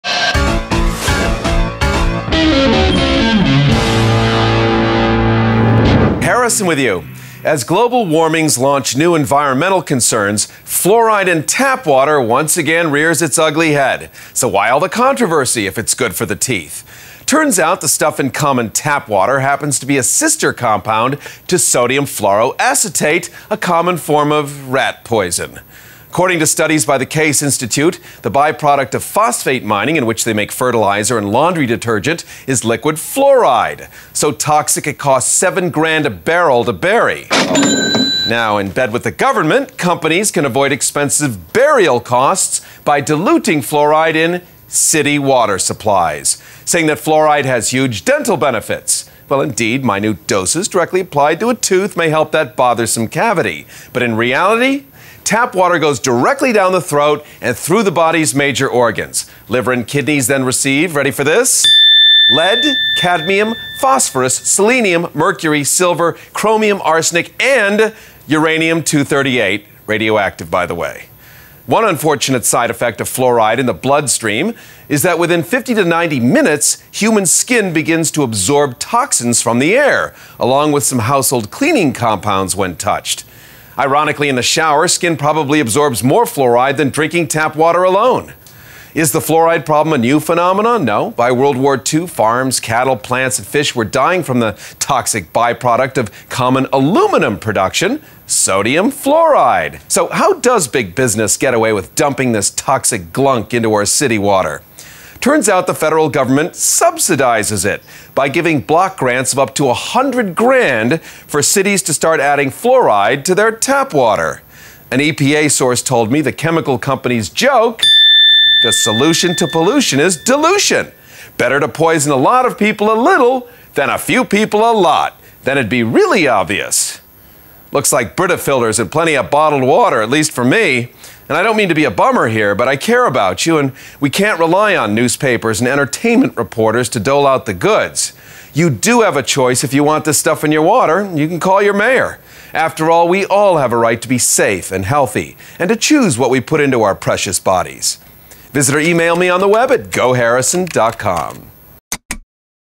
Fluoride in Water: Study Says Don't!
Why is this industrial waste product poured into city water supplies?  Who's behind it, and what are the long-term physical effects to you?